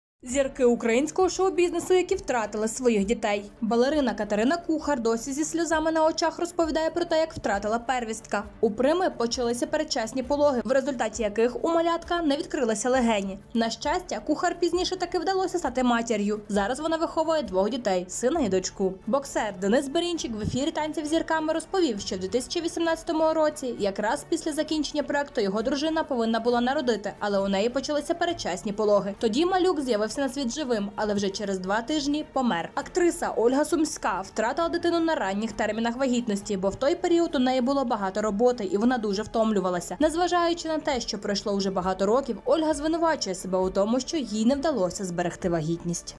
😢Зірки українського шоу-бізнесу, які втратили своїх дітей
Зірки українського шоу бізнесу, які втратили своїх дітей. Балерина Катерина Кухар досі зі сльозами на очах розповідає про те, як втратила первістка. У прими почалися передчасні пологи, в результаті яких у малятка не відкрилися легені. На щастя, Кухар пізніше таки вдалося стати матір'ю. Зараз вона виховує двох дітей - сина і дочку. Боксер Денис Берінчик в ефірі "Танців з зірками" розповів, що в 2018 році, як раз після закінчення проекту, його дружина повинна була народити, але у неї почалися передчасні пологи. Тоді малюк з'явився на світ живим, але вже через два тижні помер. Актриса Ольга Сумська втратила дитину на ранніх термінах вагітності, бо в той період у неї було багато роботи і вона дуже втомлювалася. Не зважаючи на те, що пройшло уже багато років, Ольга звинувачує себе в тому, що їй не вдалось зберегти вагітність.